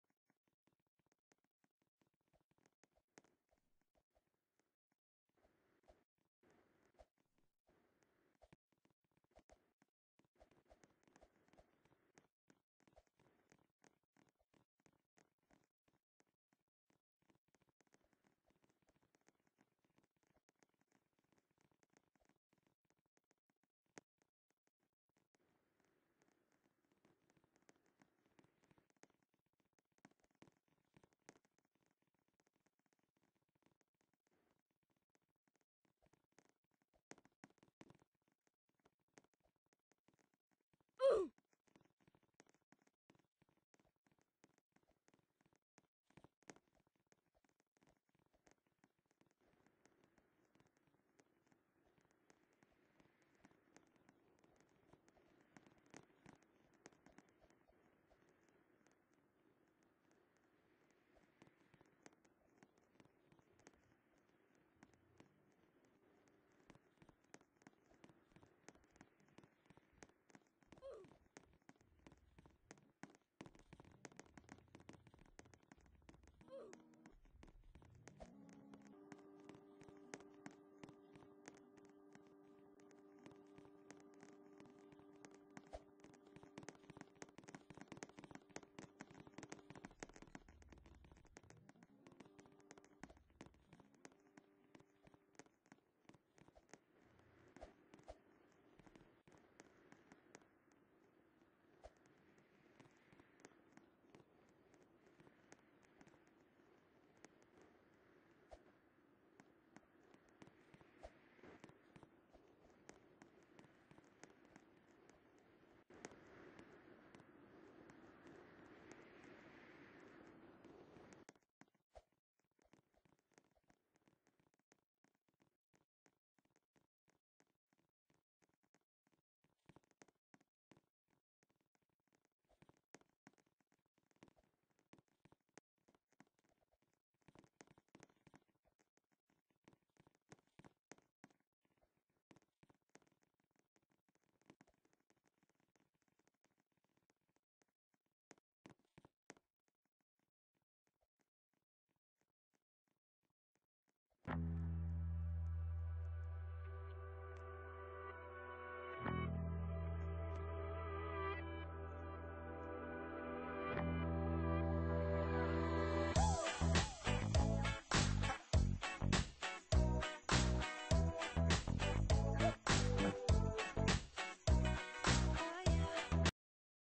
Cuff abuse and segway abuse at Hilton Hotels! | Hilton Hotels series #3
Username:heyguysitstodd abusing security tools.